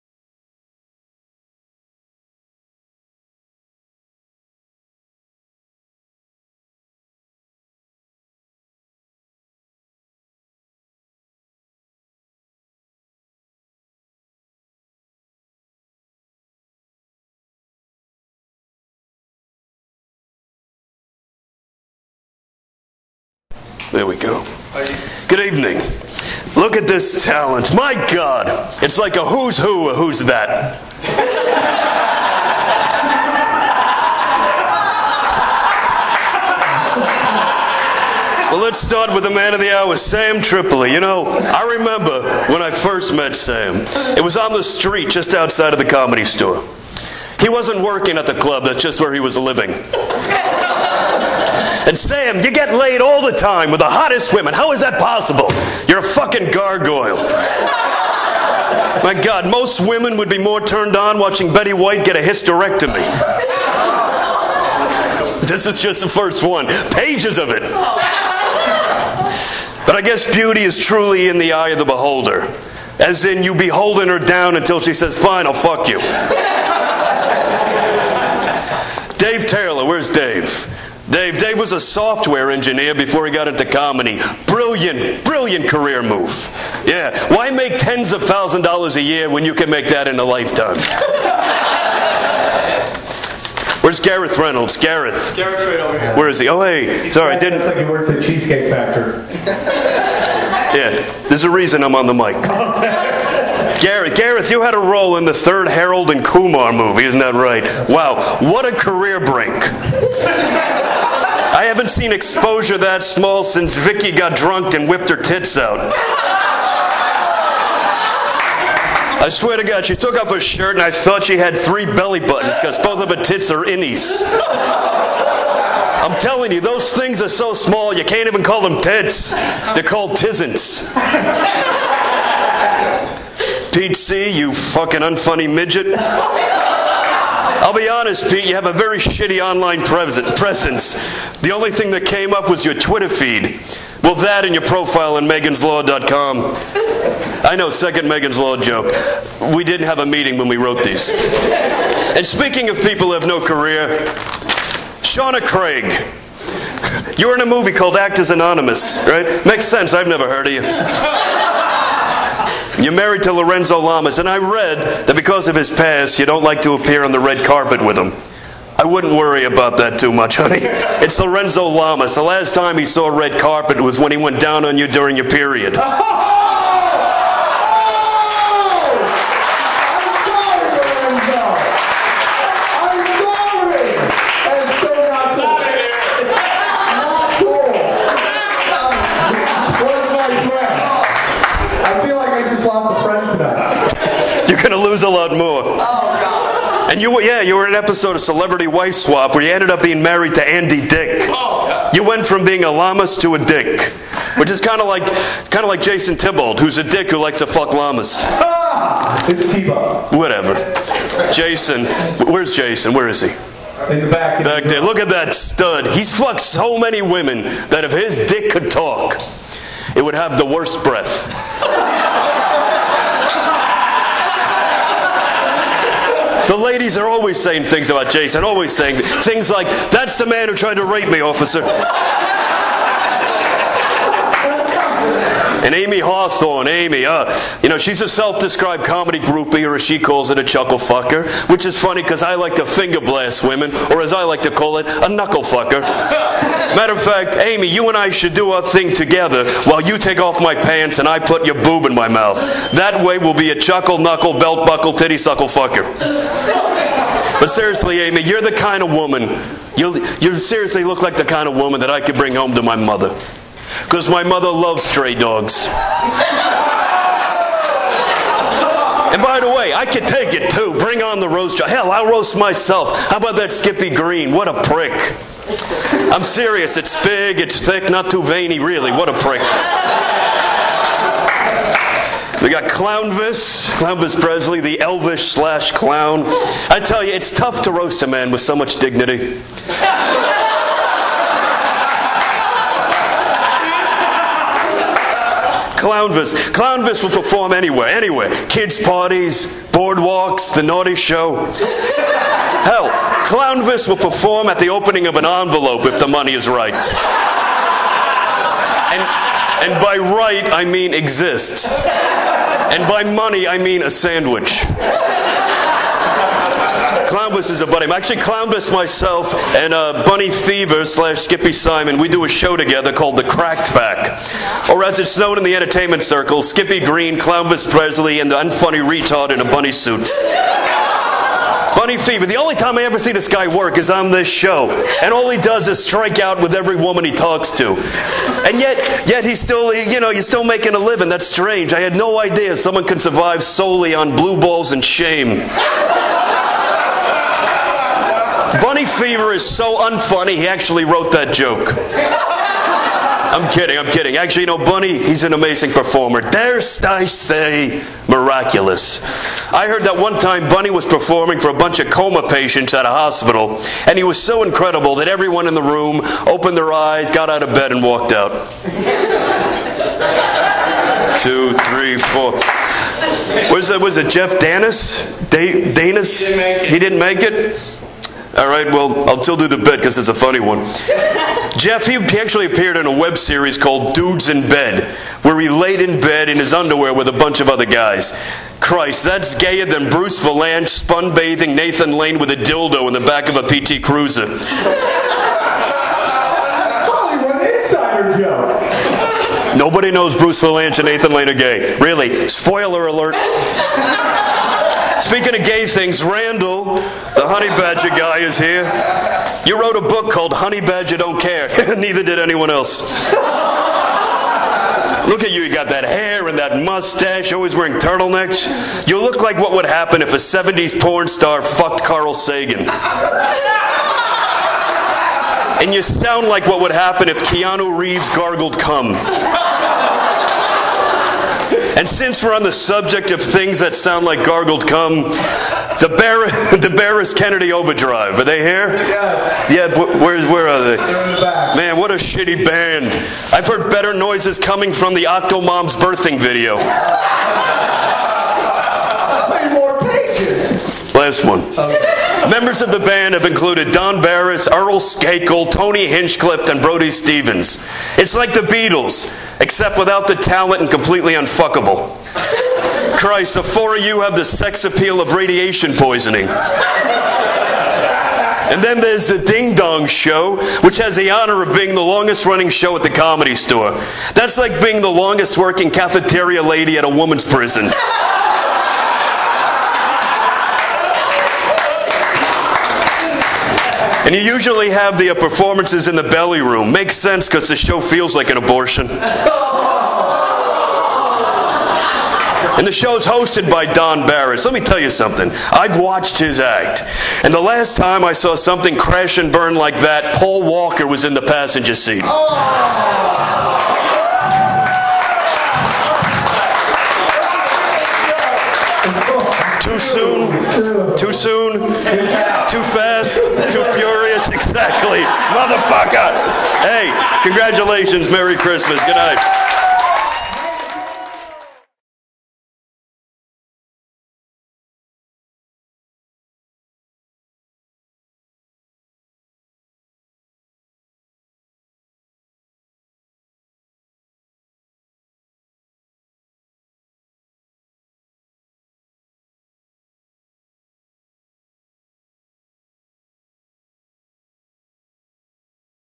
Skippy Greene performs at The Naughty Show's Roast of Samta Claus
Skippy Greene roasts several people at The Naughty Show's 2013 Roast of Samta Claus